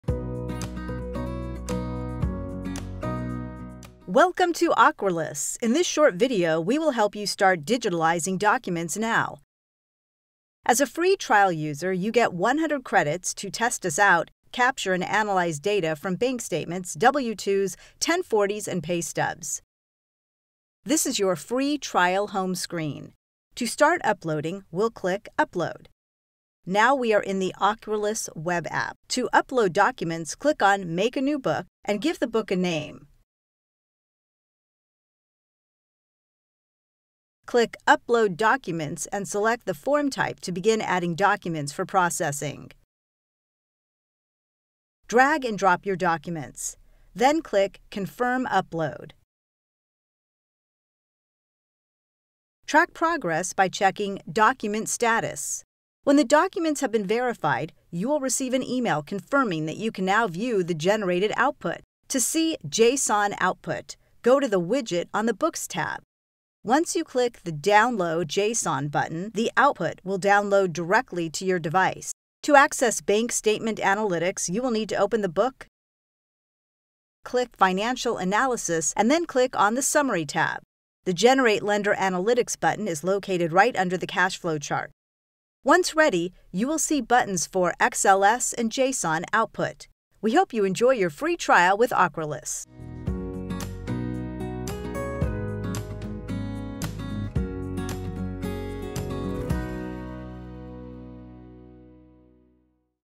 Ready to Get Started with Ocrolus Automation in Less Than 2 Minutes? Discover Our Advanced Solution
- Upload documents
- View your generated output

In this short video you’ll get a step-by-step guide of navigating from your home screen to uploading documents, tracking the document status, viewing the JSON output, and accessing bank statement analysis.

- Get full access to 100+ form types
- Discuss API integrations
- Create custom analytics